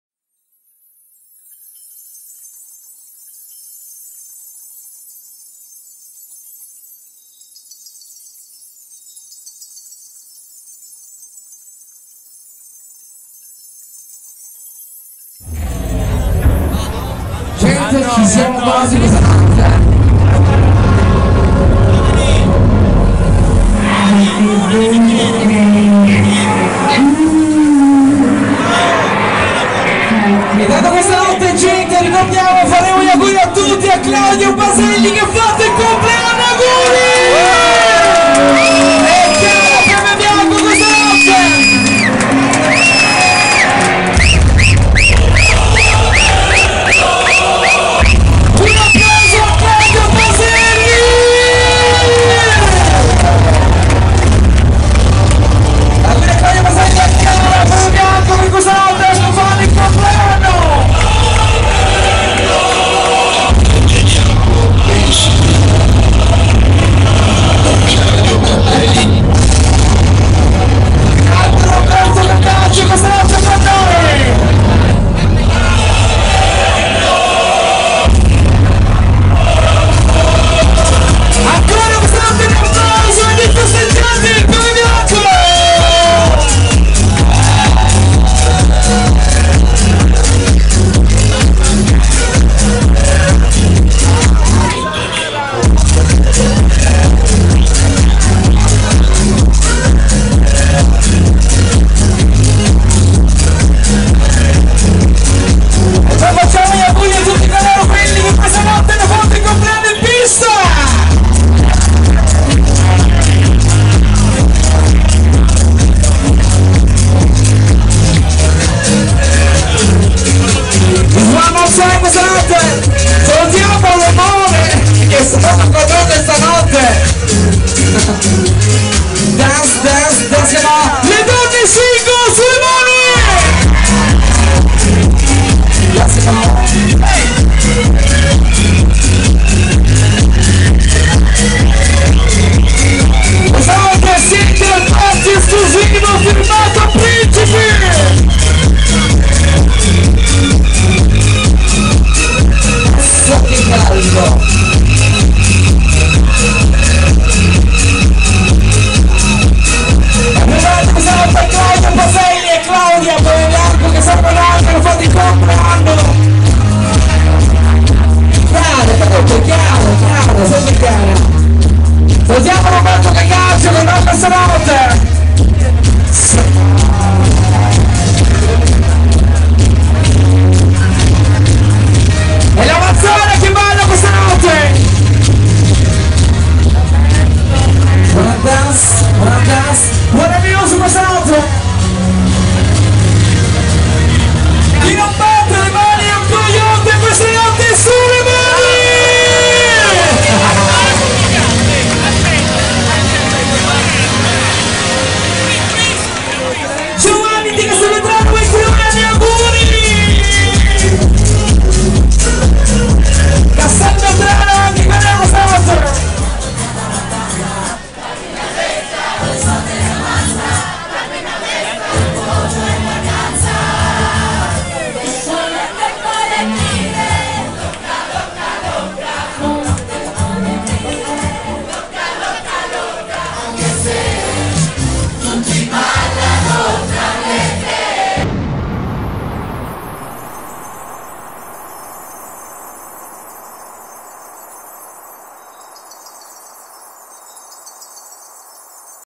PEPEBIANCO PRINCIPI Disco Glam Club V 13 Compleanno Claudio Paselli & Chiara Pepe 19 febbraio
PEPEBIANCO PRINCIPI Disco Glam Club a Mazara del Vallo

LuDj
Stefano Amico Dj
Voice Max Rocchetti
Claudio Paselli
Roberto Cacace

Lungomare Mazzini,29

per info

cell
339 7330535